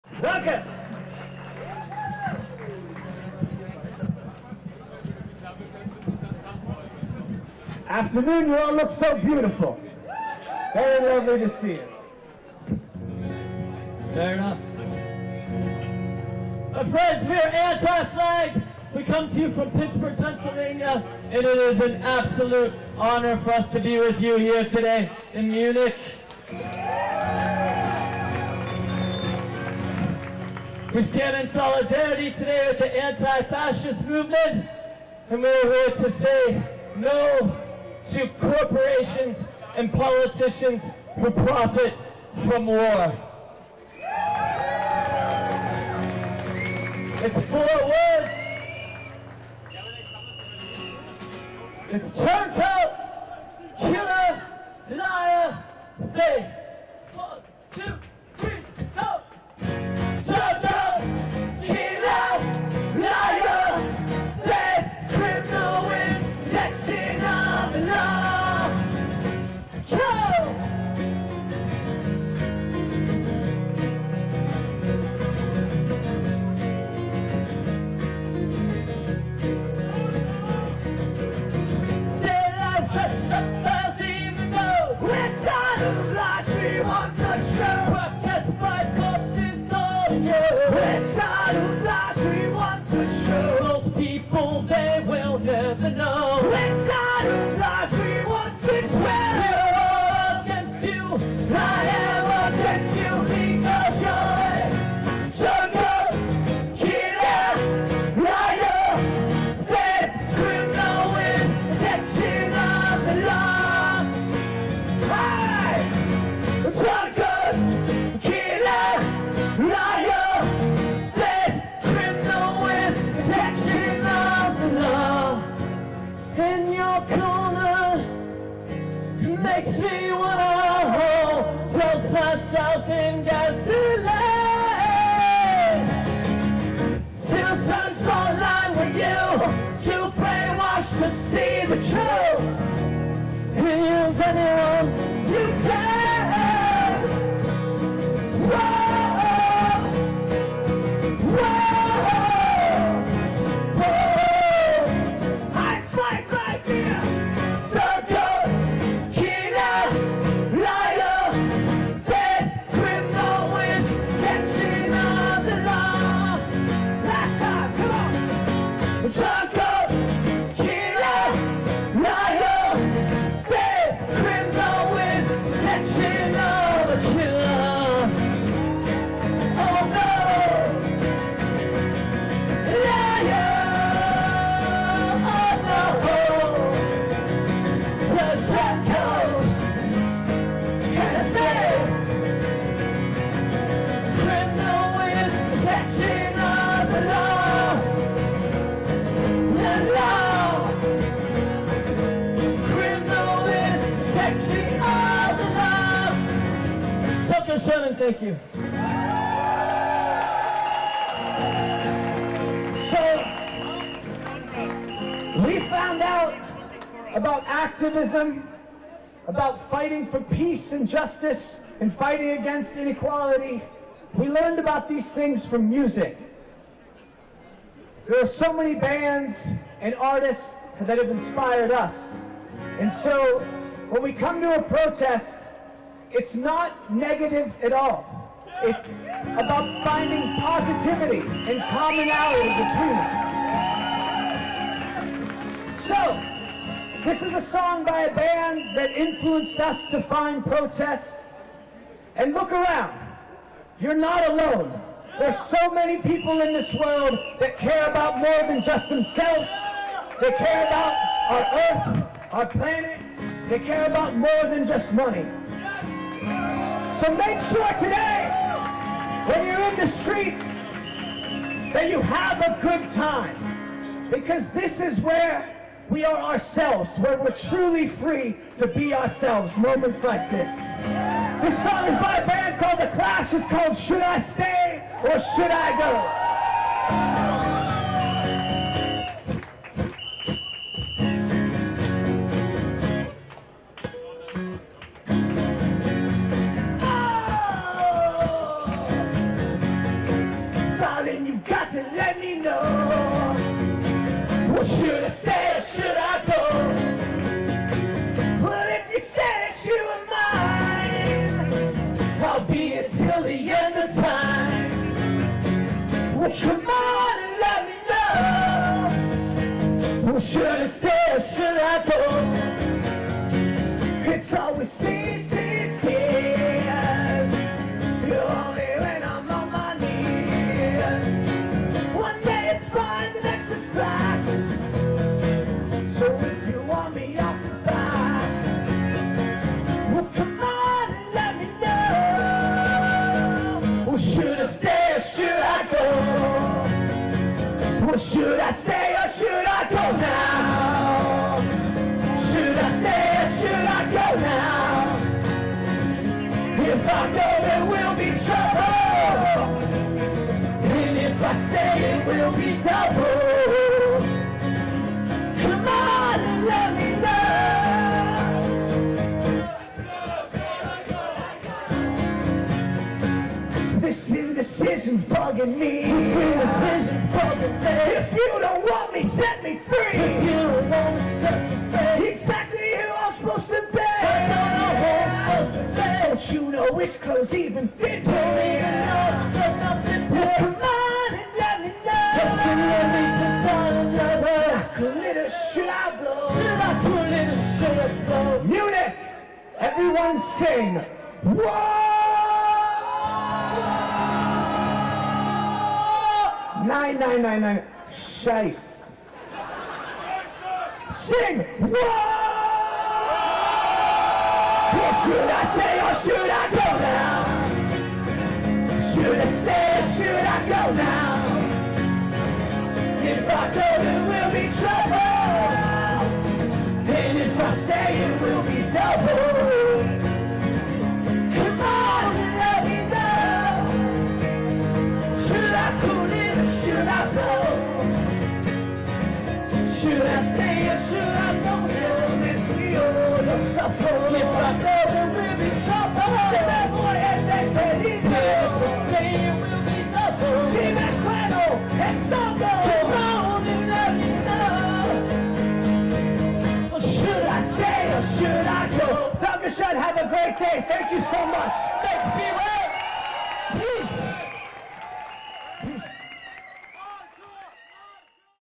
Justin Sane + Chris#2 von ANTI FLAG /USA Anti-Siko 13.2.2016
Ein  musikalischer Höhepunkt  bei der Veranstaltung gegen die Münchner Sicherheitskonferenz waren Justin Sane + Chris#2 von der Polit-Punk Band ANTI-FLAG aus Pittsburgh/USA. Viele der Kundgebungsteilnehmer waren sehr inspiriert und sangen ihre Songs mit.